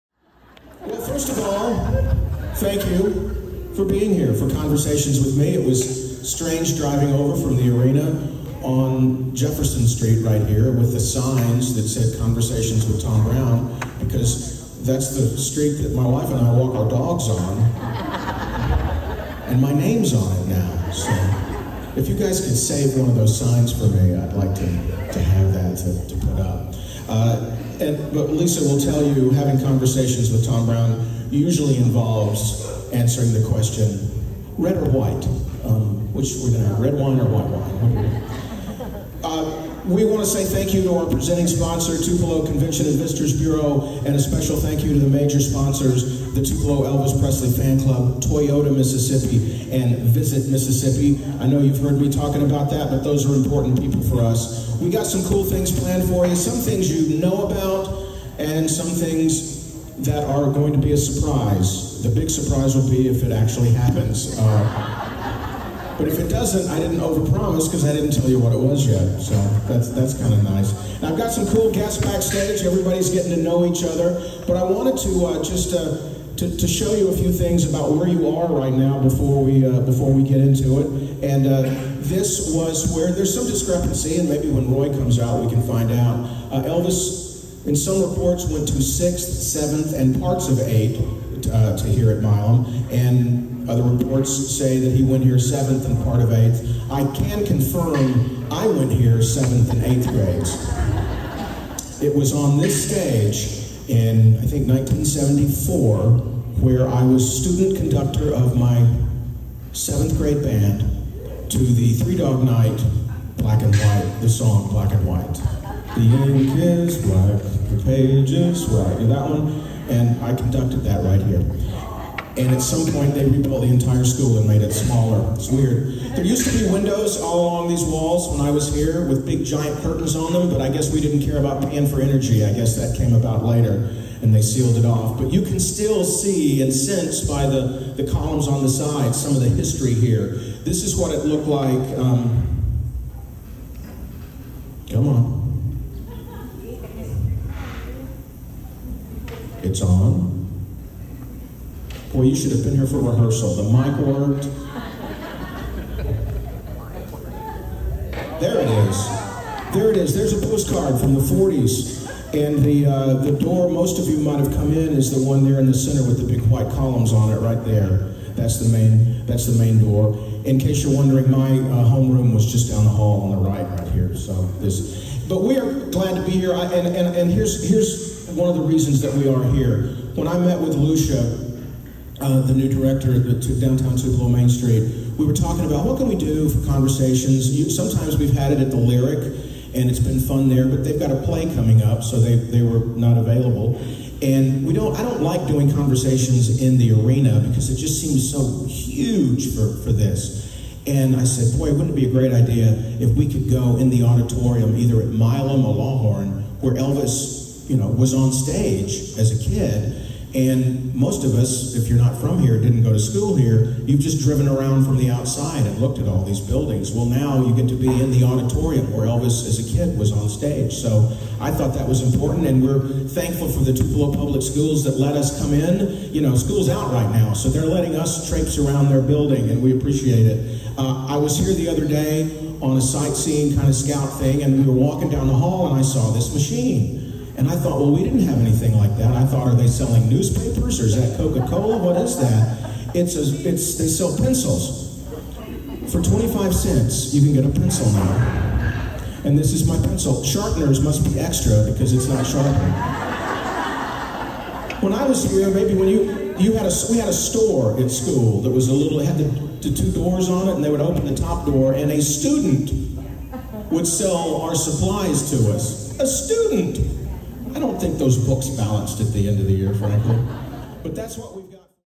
Conversations with Tom Brown intro - video by Susan Quinn Sand
Conversations with Tom Brown at Milam Elementary School, Tupelo, MS, 10 June 2022